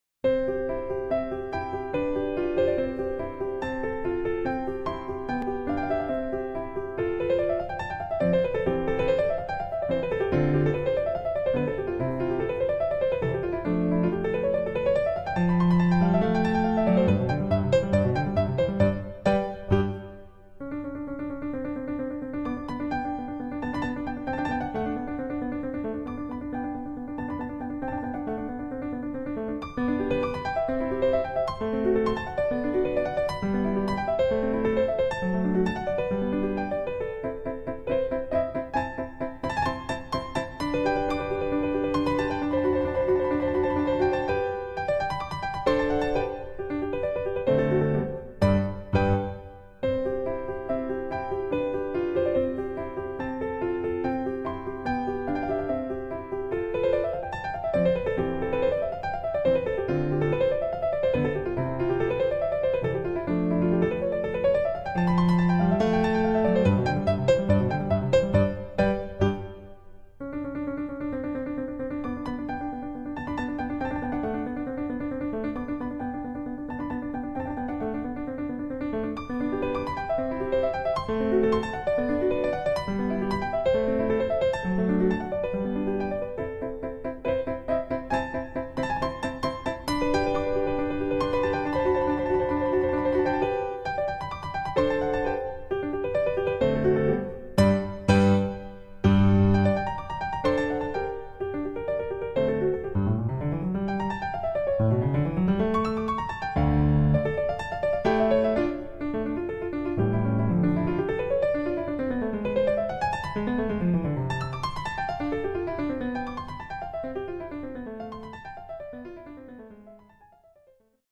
LGBTQ+ Canon Characters - NightStarverse - Nathaniel Kurtzberg (Speedpaint)
In my AU Nathaniel Kurtzberg (Miraculous: Tales of Ladybug and Chat Noir) is Biromantic-Asexual.